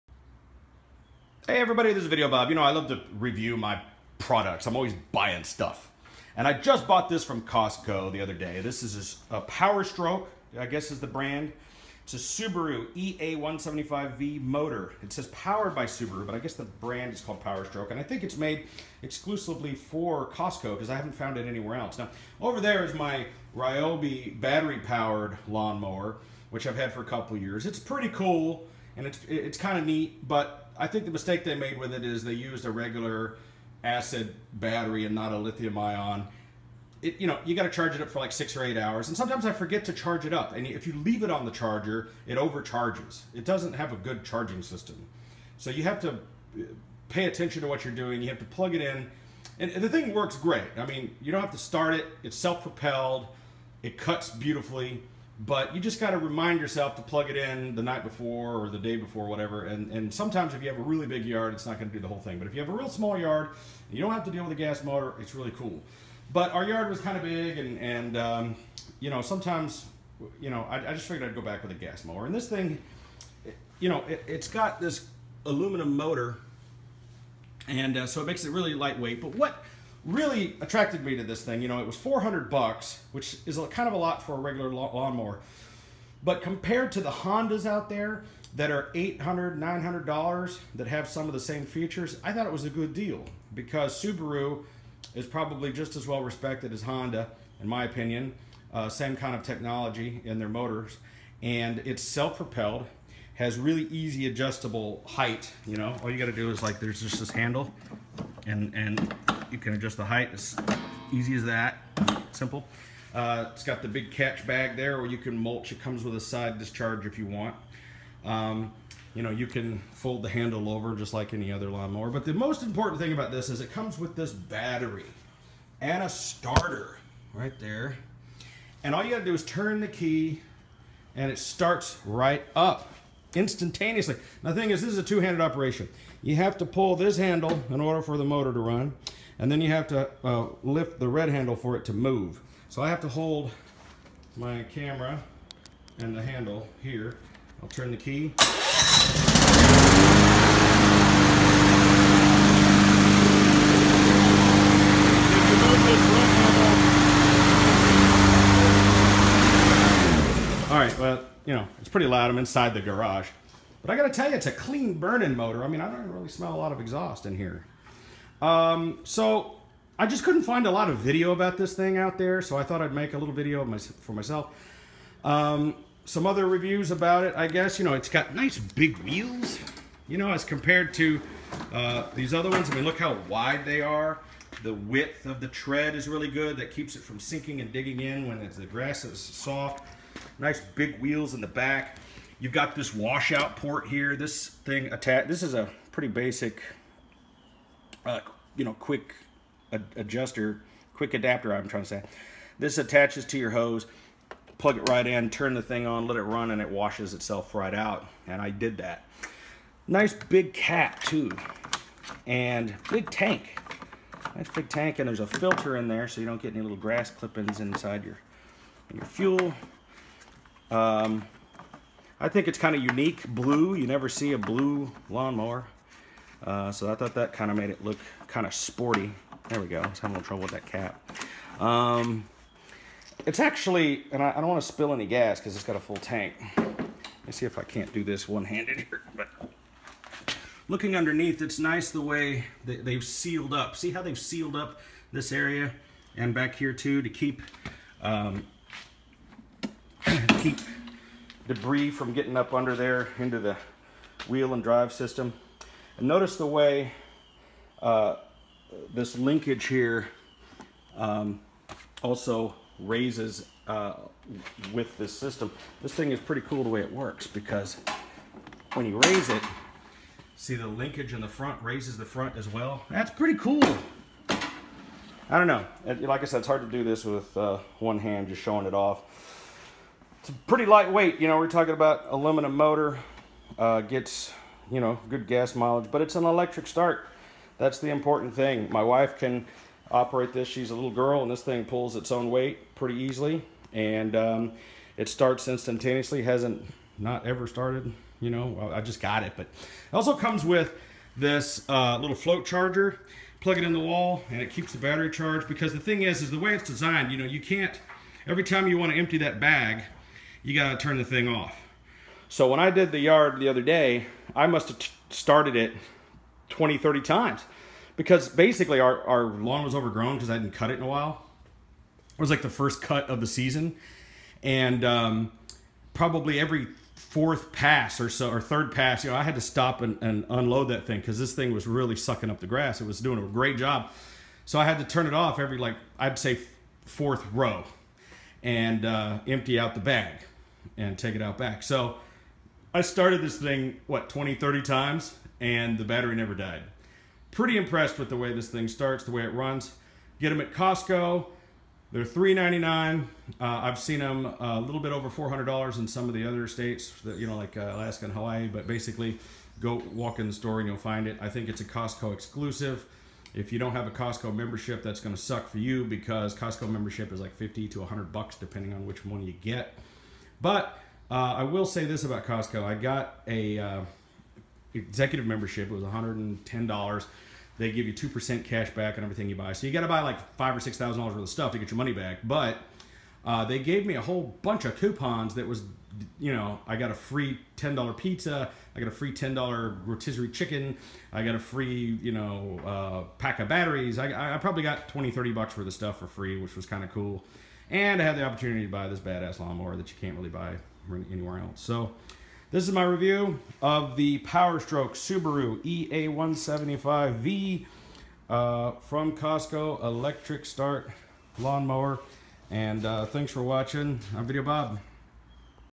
Powerstroke Subaru EA175V Electric Start Self Propelled Lawn Mower by Costco
This is a Costco exclusive, the Powerstroke lawnmower is powered by an aluminum cast Subaru EA175V gas engine.
Features a built in battery and electric start mower, self propelled large rear wheels, rear bagging or mulching, built in hose adapter for wash out and easy adjustment of all 4 wheels with one big easy to use handle. Best lawnmower you can get for $400 no question about it!